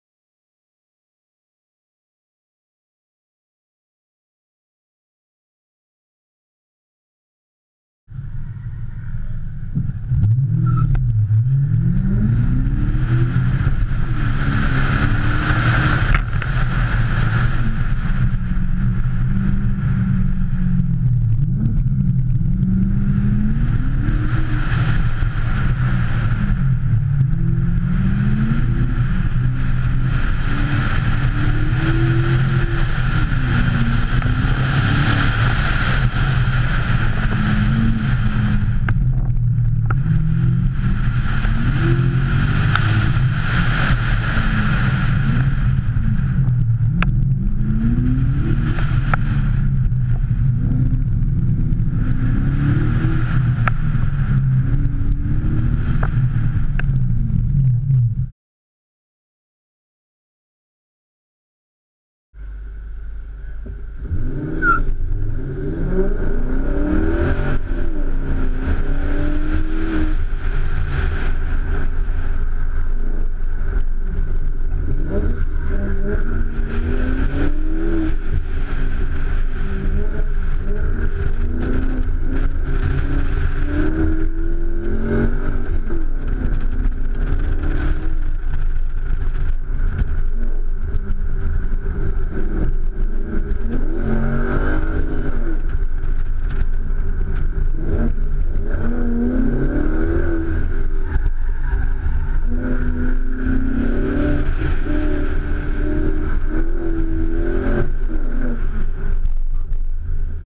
First Auto x
This is footage from the first ever auto-x i've ever participated in. I've got along ways to improve. I was driving a BMW 645ci. There are two runs.